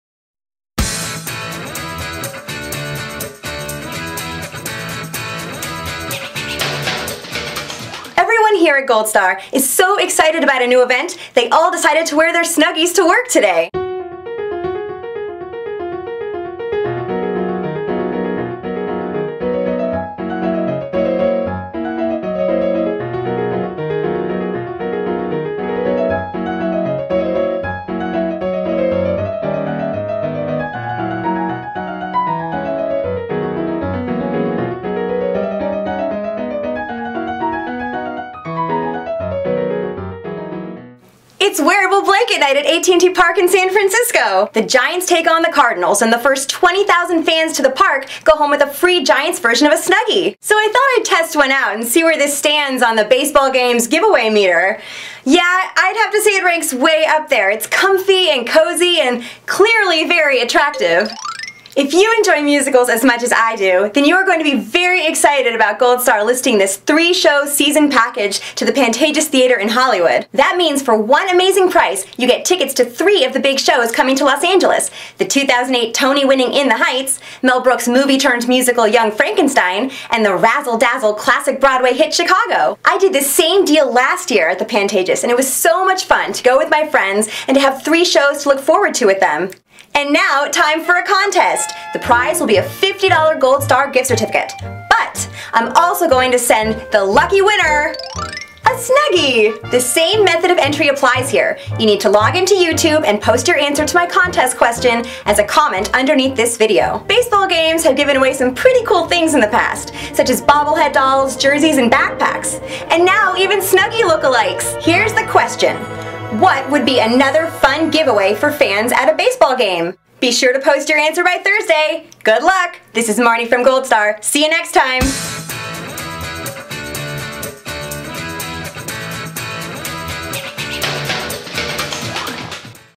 GOLDSTAR WEEKLY - WEEK OF APRIL 12, 2010
A quick look at a few events on Goldstar hosted by Marni Landes. Topics of the week: Goldstar staff wear Snuggies to work, wearable blanket night with the San Francisco Giants, three show package for the Pantages in Hollywood and of course, another chance to win a $50 Goldstar Gift Certificate...  AND a Snuggie!. Contest deadline is Thursday, April 15, 2010.